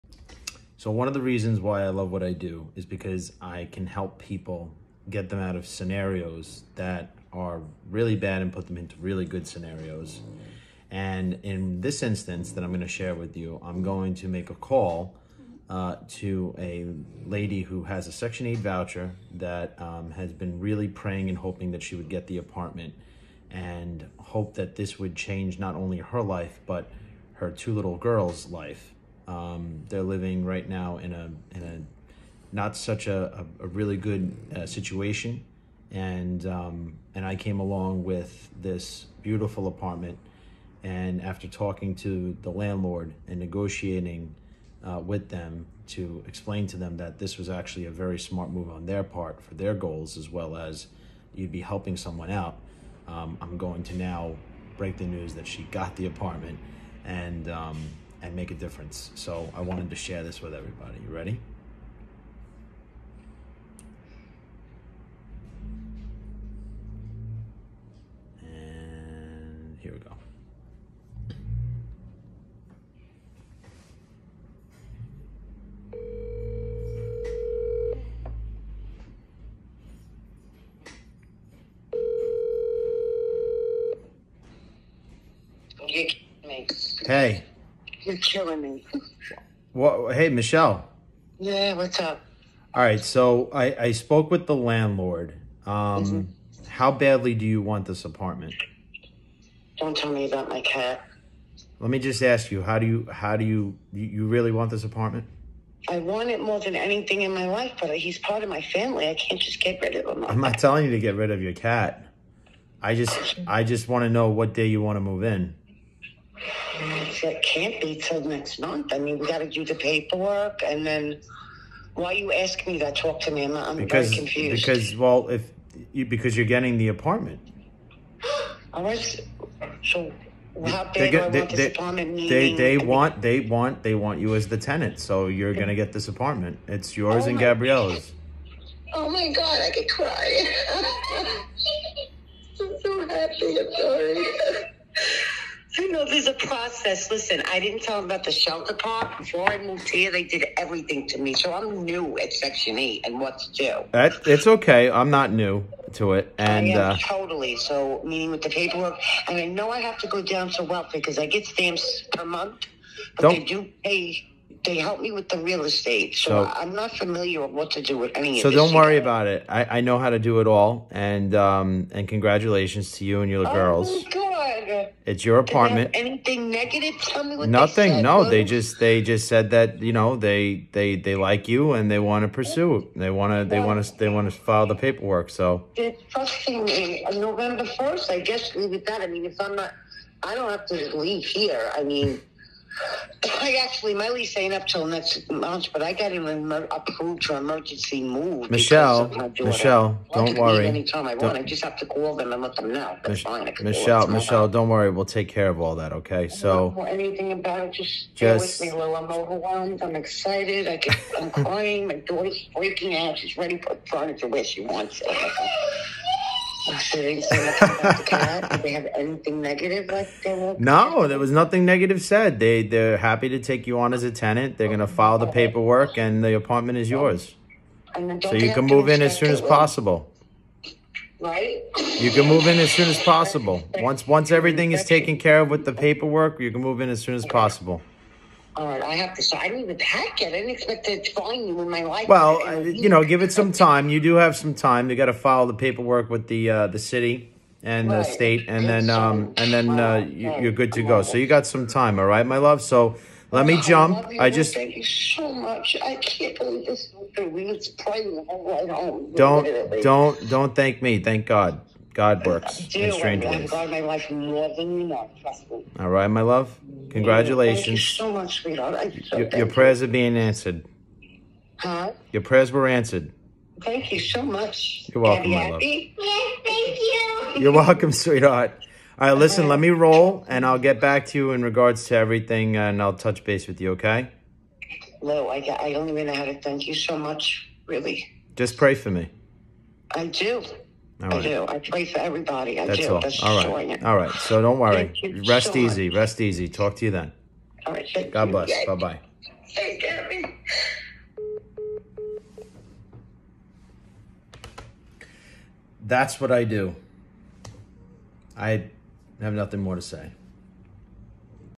How I helped change one special woman and her 2 little girls lives. That’s what I ❤️ to do.
In this video I break the news to a woman who desperately needed a miracle that she had been excepted for the apartment that she was hoping to get for both her and her two daughters. After many years of despair and distress and misfortune she now has a home where she feels comfortable to start all over again and raise her family. All I did was negotiate on gods behalf to help this lady and her children to find an apartment for them so they can start a new life. This is why I love what I do.